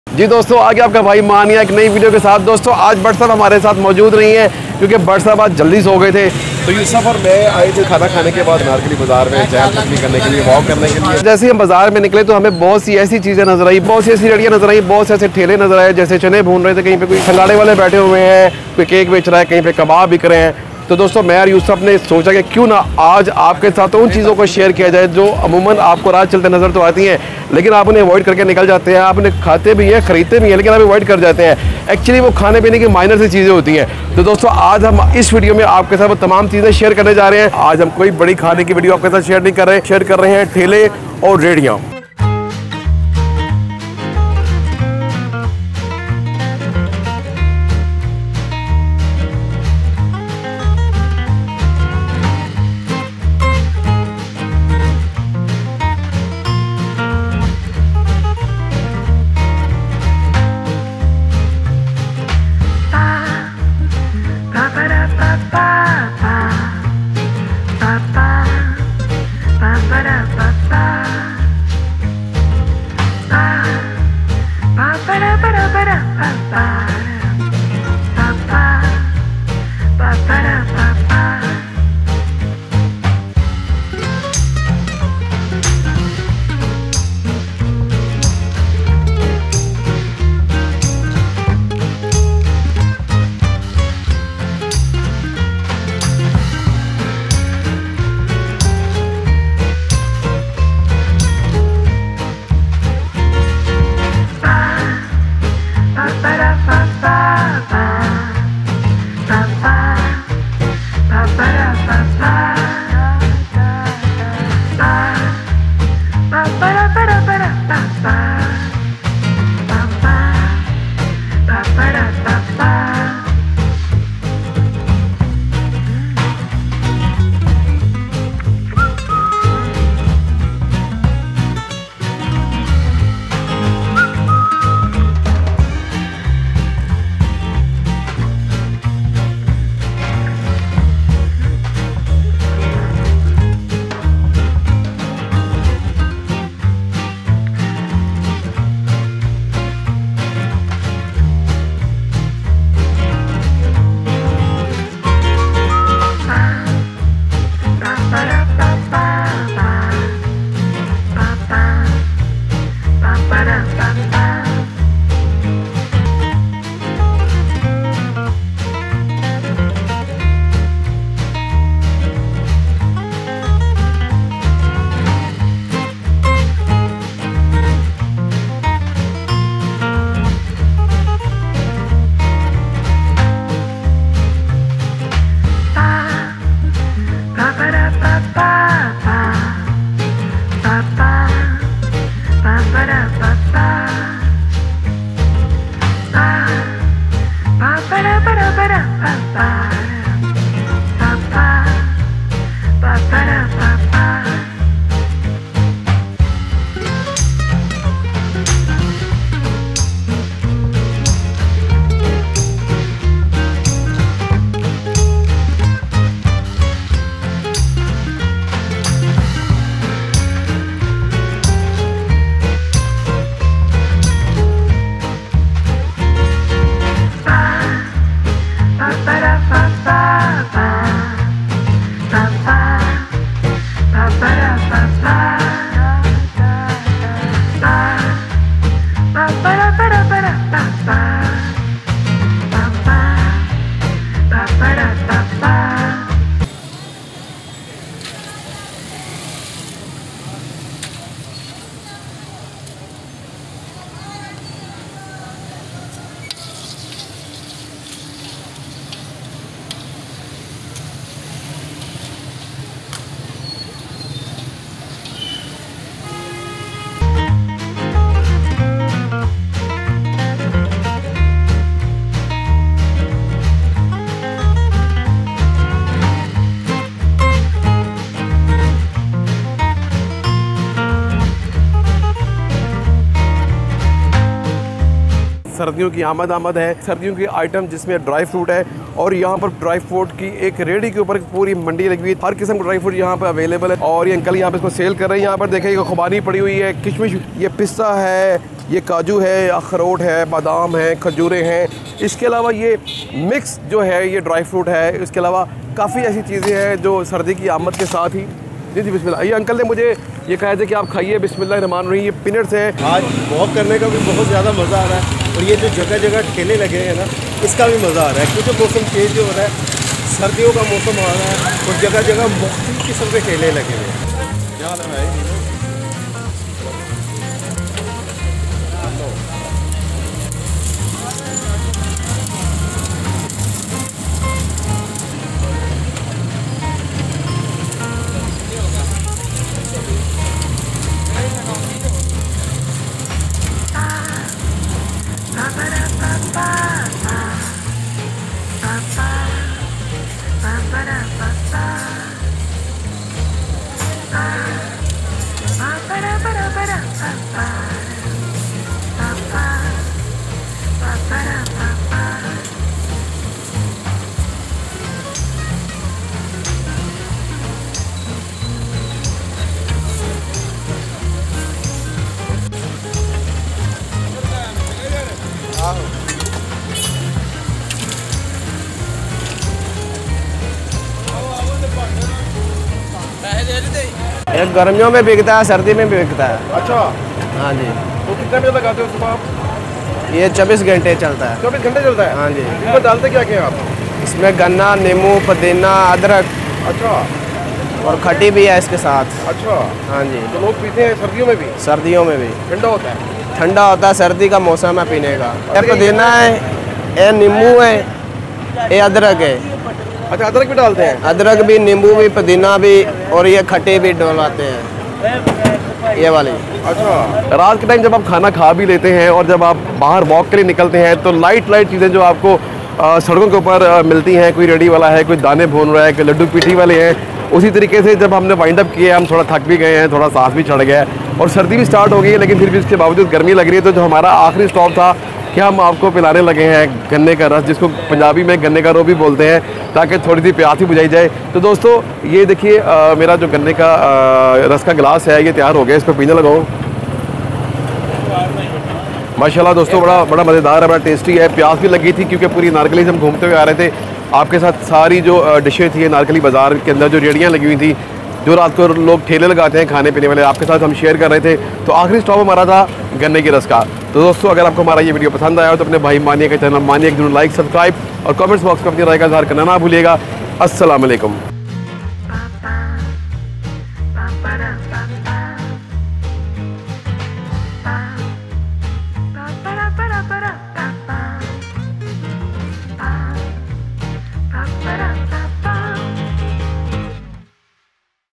Road Side Random Food | Street Food | Food Secret | Pakistani Street Food
Hi Friends
In This Video Mania Aik Junoon Sharing with you Road Side Random Food in street food of pakistan. Friends Lahore is historical city of Pakistan. in this video we visit late night anarkali bazar, and we found amazing different kind of small food stalls in this bazar.
Small Food Stalls we discover for you in this video;
1. Road Side Dry Fruit Stall
2. Roadside Laddu Peethi Stall
3. Roadside beautiful Bakery
4.  Roadside Chestnuts, Sangharay,
5. Roaside Beaf Kabab
6. Roadside Sugarcane Juice
7. Roadise Samosa Stall
8. Roadside Coconut Stall
9. Roadside Chana Chaat
I Hope you will enjoy this video;
Apka Bhai
Mana Aik Junoon